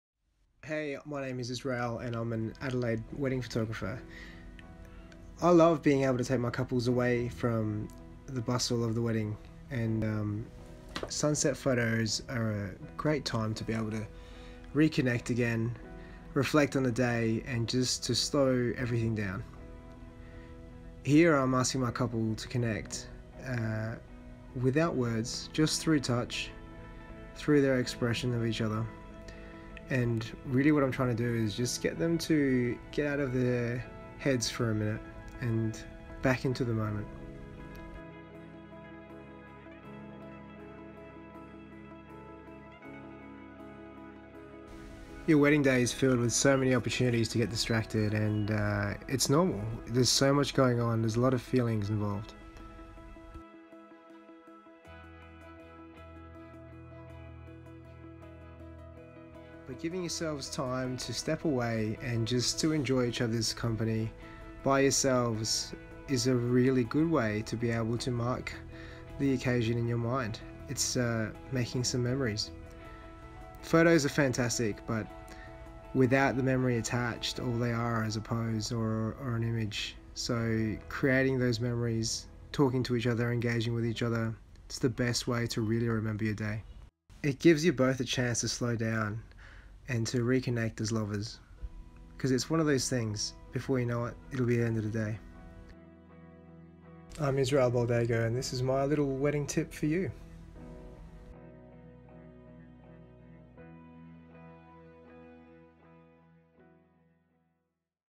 **Wedding Tip** Why you shouldn't just get "photos" taken on your wedding day
A little behind the scenes video recorded on my iphone about why getting away for some alone time during your wedding is important.